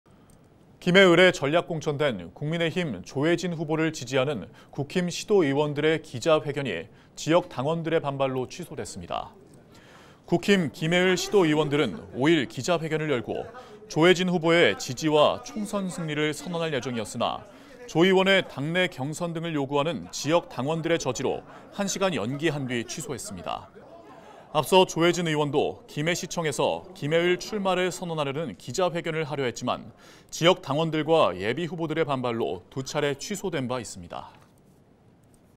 국힘 시·도의원, 김해을 '조해진' 지지 기자회견 취소…국힘 지역 당원 강력 저지ㅣ#헬로tv뉴스
김해을에 전략공천된 국민의힘 조해진 후보를 지지하는 국힘 시·도의원들의 기자회견이 지역 당원들의 반발로 취소됐습니다.